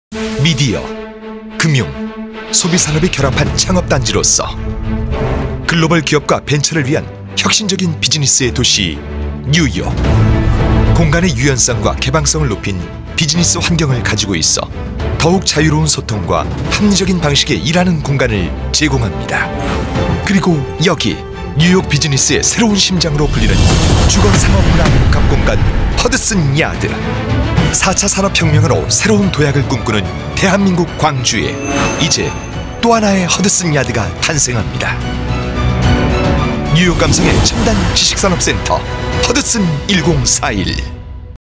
첨단2지구 지식산업센터 허드슨 1041 홍보영상_1
첨단2지구 허드슨 1041
지식산업센터 및 지원시설 분양

광주광역시 북구 연제동 1041번지
지하3층 ~ 지상15층, 연면적 37,879㎡

평당5백만원대로 저렴한 분양가
최대 80% 장기 저리융자 가능
부동산 규제 無, 전매제한 없음

IT∙스타트업이 대거 밀집한 뉴욕의 ‘워크 앤 라이프’를 컨셉
뉴욕의 ‘허드슨 야드’의 분위기와 디자인을 도입하여 비즈니스와 삶이 공존하는 허드슨 야드 라이프스타일을 구현

오피스 층고 최대 5.4m로 우수한 개방감(복층 설계 가능)
지식산업센터내 뉴욕 공유오피스를 벤치마킹한 공유오피스 등 커뮤니티 시설
옥상정원과 완충 녹지를 활용한 공원(예정) 등의 휴식 공간
회의실, 세미나실, 상업시설 등 각종 편의시설
삼성전자 비즈니스 사물인터넷(b.IoT)이 적용

함께 조성되는 상업시설의 경우 뉴욕의 자유분방하고 세련된 분위기를 느낄 수 있는 디자인 ‘브릭 앤 메탈’ 스타일의 스트리트몰로 설계
상업시설 1층에 미디어 파사드를 설치하여 문화 예술이 결합된 복합 공간 조성을 통해 집객력 강화
(락볼링장, 대형 피트니스 입점 예정)
100% 자주식 주차시설 완비

(주)황소 (주)황소부동산중개법인

광주, 전남권 부동산 매물접수
분양대행 / 컨설팅 / 임대관리 및 매매